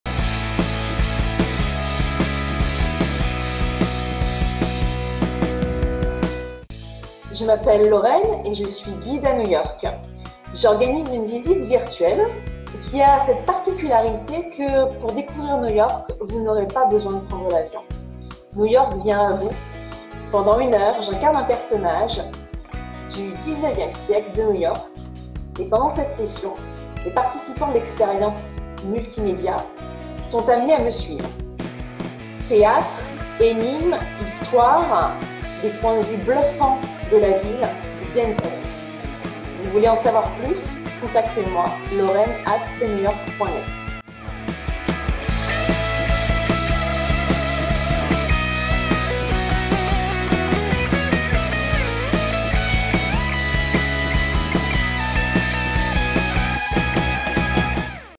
Participez à notre visite virtuelle de New York et remontez le temps !
Laurène Hamilton, guide certifié de New York, vous transporte au 19ème siècle... Une visite virtuelle inédite à découvrir